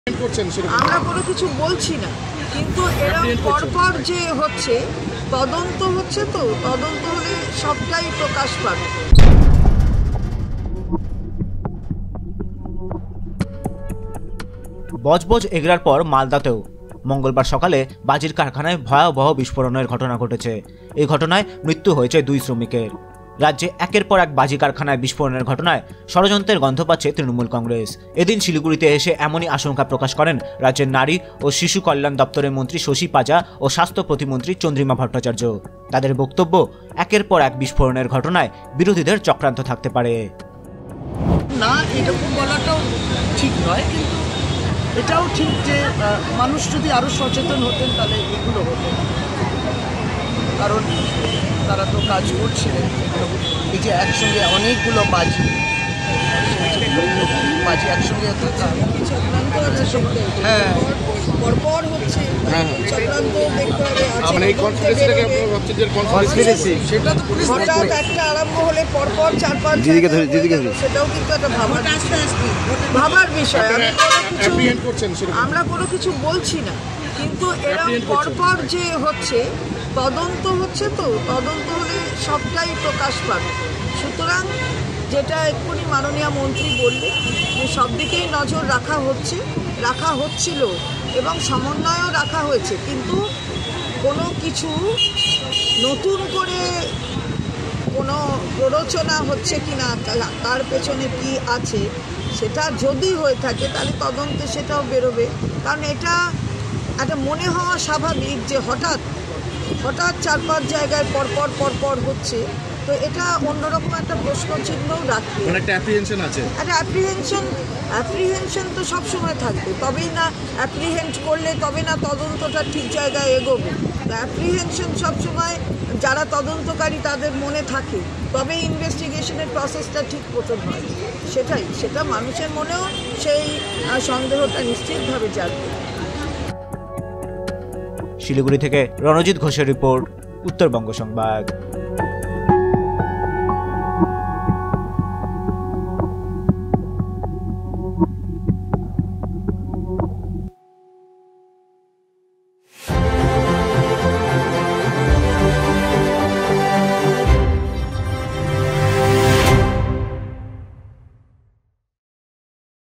BREAKING | বিরোধীদের চক্রান্তেও হতে পারে বিস্ফোরণ, মন্তব্য শশী পাঁজার
রাজ্যে একের পর এক বাজি কারকানায় বিস্ফোরণের ঘটনায় ষড়যন্ত্রের গন্ধ পাচ্ছে তৃণমূল কংগ্রেস। এদিন শিলিগুড়িতে এসে এমনই আশঙ্কা প্রকাশ করলেন রাজ্যের নারী ও শিশু কল্যাণ দপ্তরের মন্ত্রী শশী পাঁজা ও স্বাস্থ্য প্রতিমন্ত্রী চন্দ্রিমা ভট্টাচার্য।